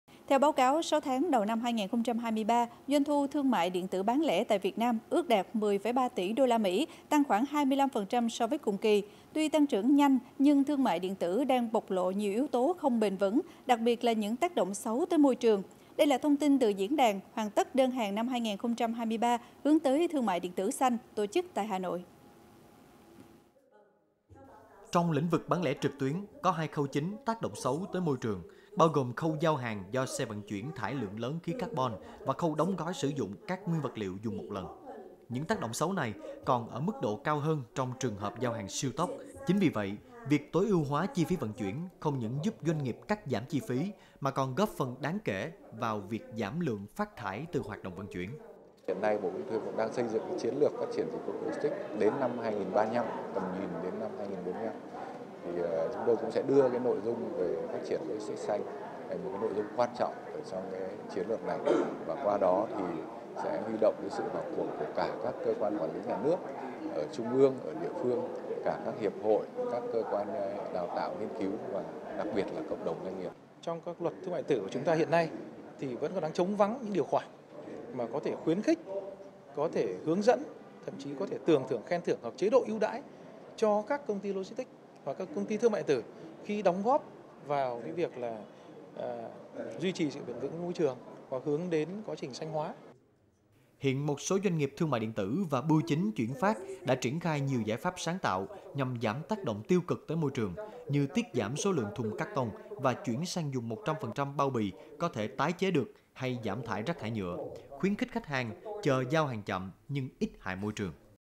PHÁT TRIỂN THƯƠNG MẠI ĐIỆN TỬ XANH
Theo báo cáo 6 tháng đầu năm 2023, doanh thu thương mại điện tử bán lẻ tại Việt Nam ước đạt 10,3 tỷ đôla Mỹ, tăng khoảng 25% so với cùng kỳ. Tuy tăng trưởng nhanh, nhưng thương mại điện tử đang bộc lộ nhiều yếu tố không bền vững, đặc biệt là những tác động xấu tới môi trường. Đây là thông tin từ Diễn đàn "Hoàn tất đơn hàng 2023 - Hướng tới thương mại điện tử xanh" tổ chức tại Hà Nội.